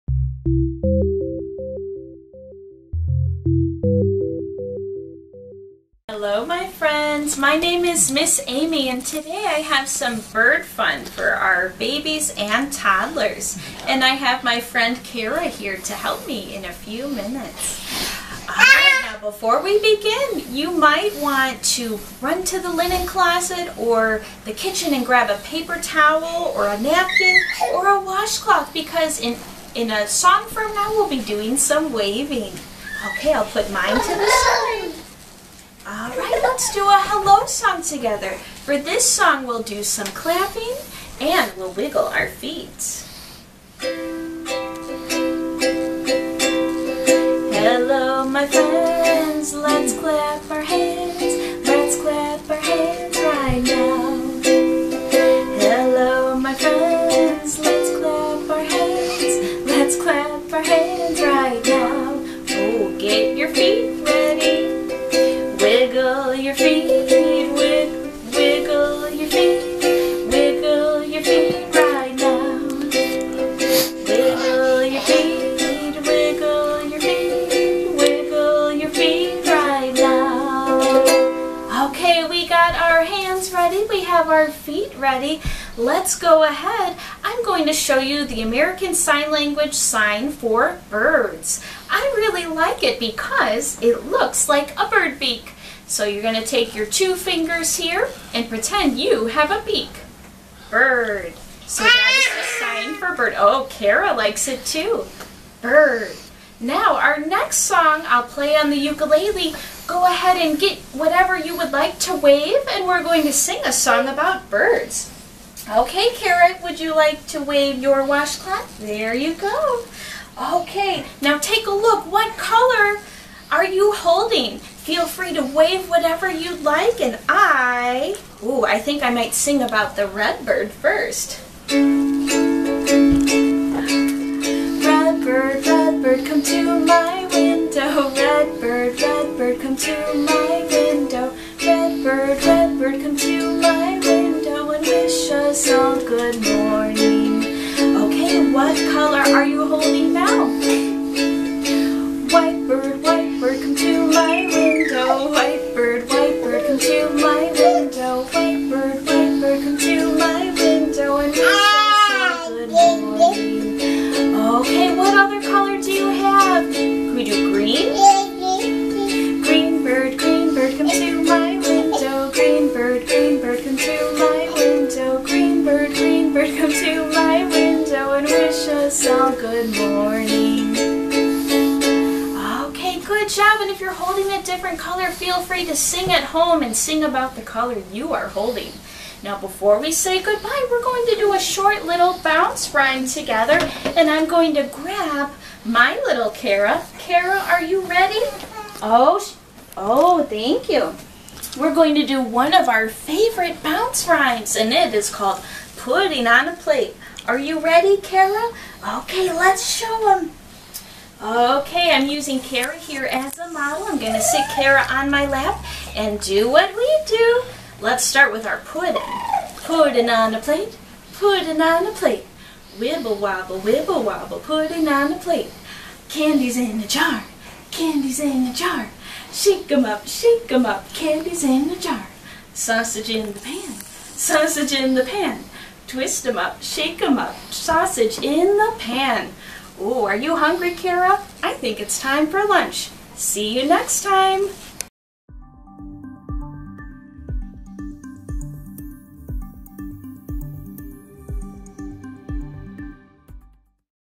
Songs for Babies with Miss Amy (Mount Prospect Public Library)
Youth songs for babies featuring Miss Amy from the Mount Prospect Public Library.